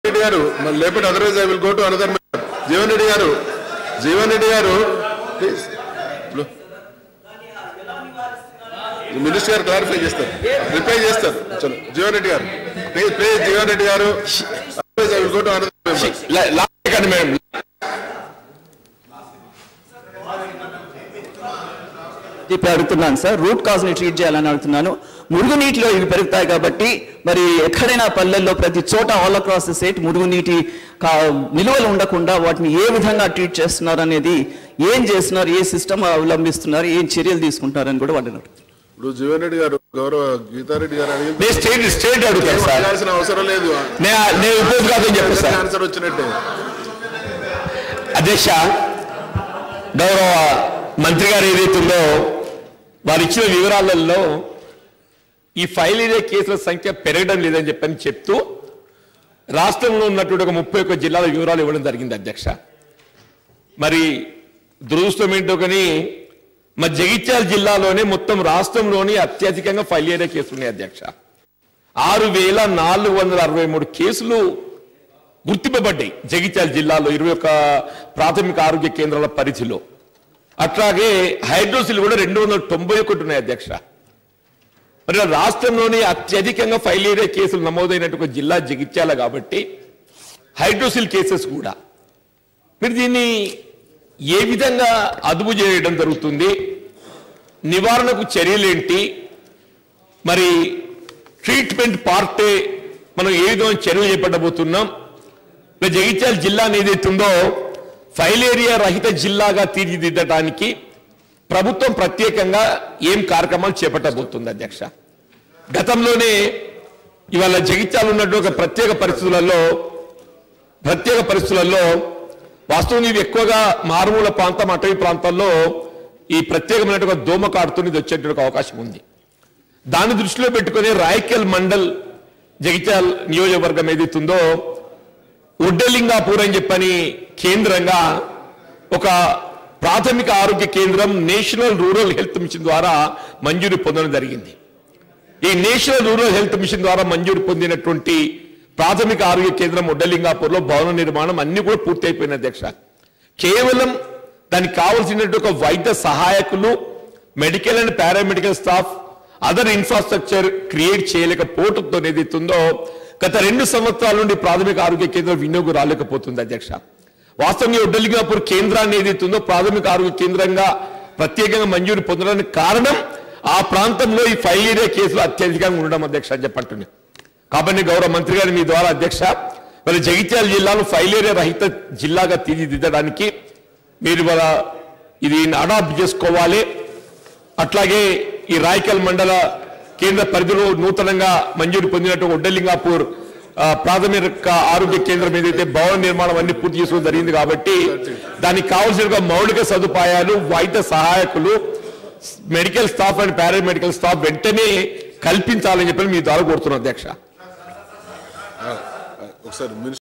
Telangana Assembly Winter Sessions | Congress MLA Jeevan Reddy Speaks On Filaria | YOYO NEWS24
Congress MLA Jeevan Reddy Speaks On Filaria
telangana Assembly Winter Sessions 2017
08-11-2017